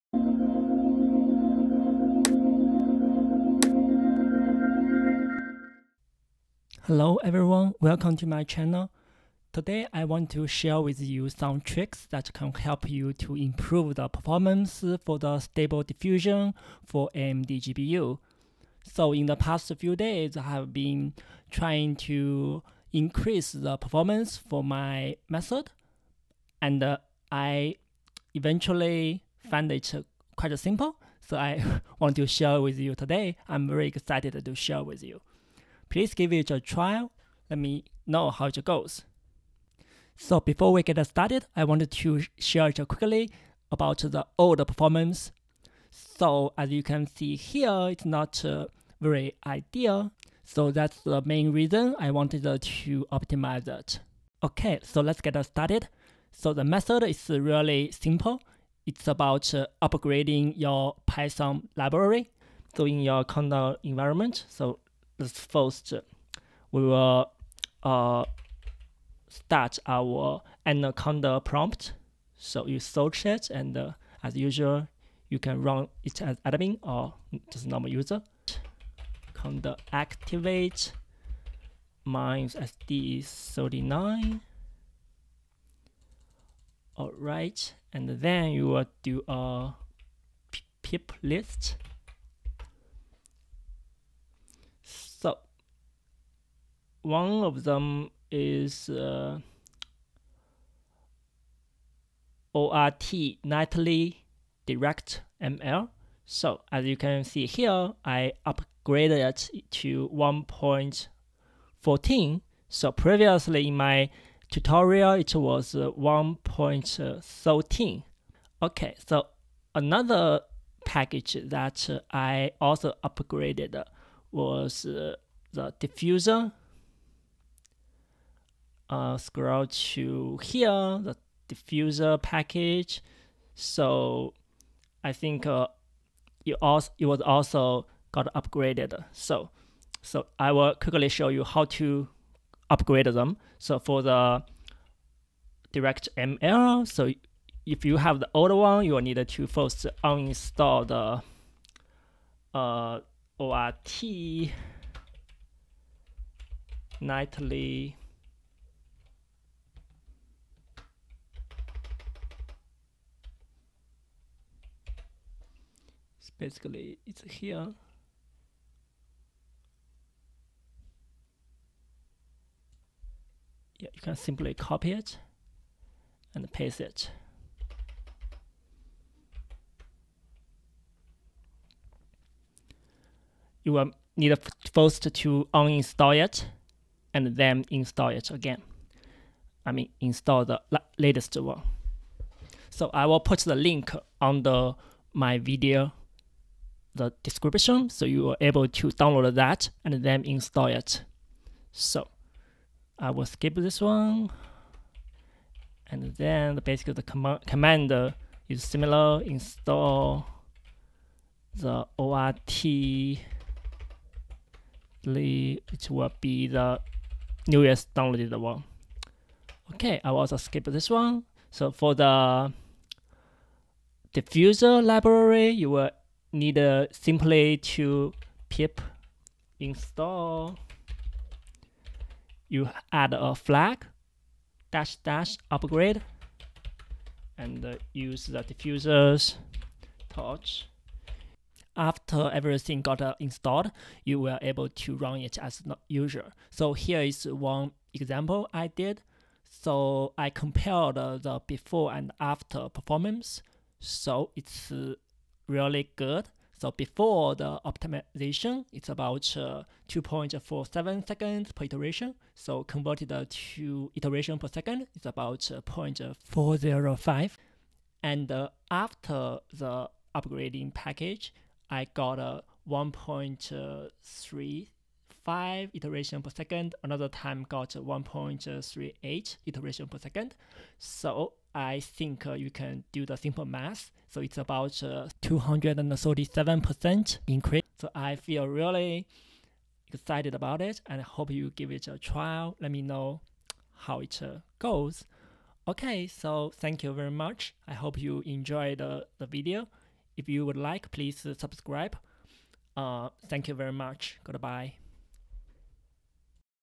AMD GPU/Windows simple tricks to speed up the stable diffusion image generating - over 200% increase
Simple tricks to speed up stable diffusion.
to generate any styled images !!
👉🏽Update: Mar 23. where you can ask questions and also try out my recently developed free chatgpt bot and stable diffusion bot!
This is a follow up to the original video of the series which contains the step by step setup.
0:47 performance before optimization
2:23 upgrade ort-nightly-directml package. The file link (make sure to select the correct python version, py39 in my case)
3:40 upgrade diffusers package. pip install --upgrade diffusers[torch]
4:10 before and after performance comparisons